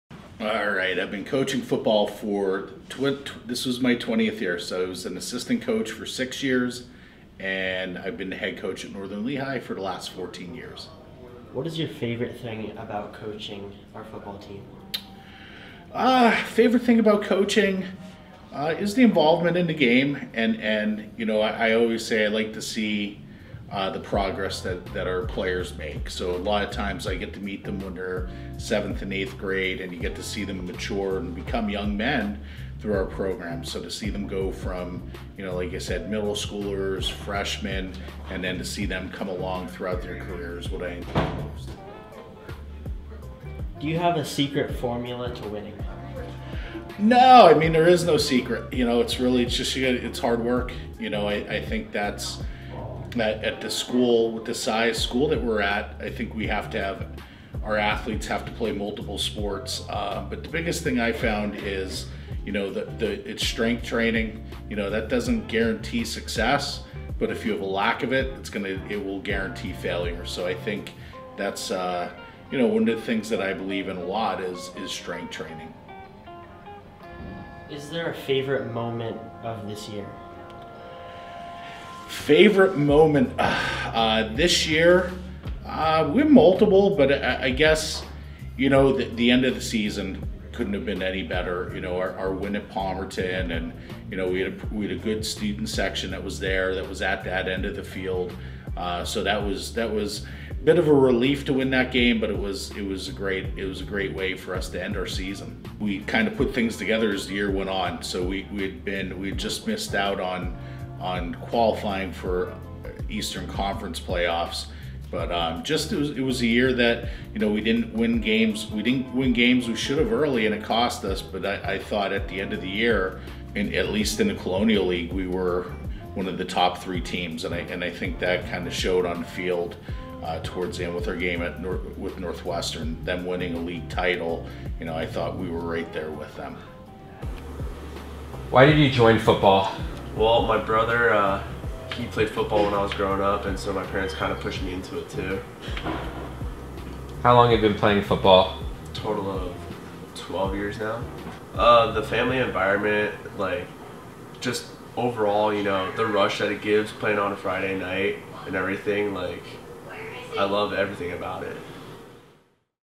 Football | 2020 Yearbook
Team Yearbook interviewed Mr. Joseph Tout and Carter Smith about Football.

2020 "Catch a Glimpse" Yearbook